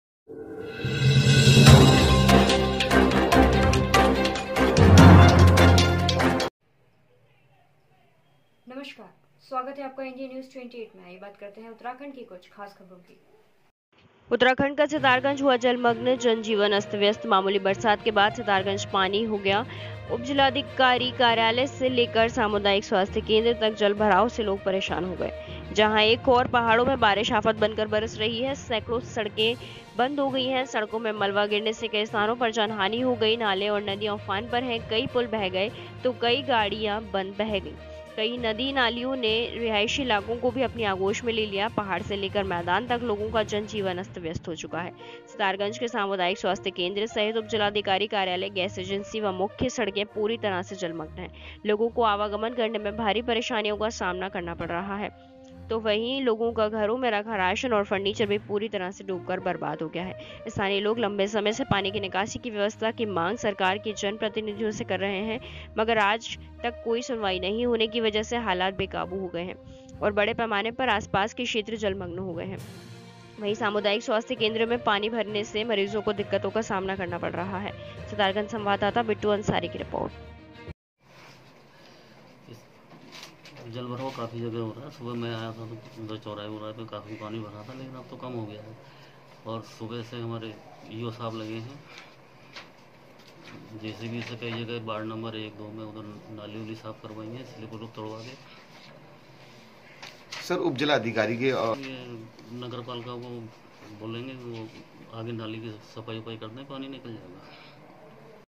उत्तराखंड का सितारगंज हुआ जलमग्न जनजीवन अस्त व्यस्त
Official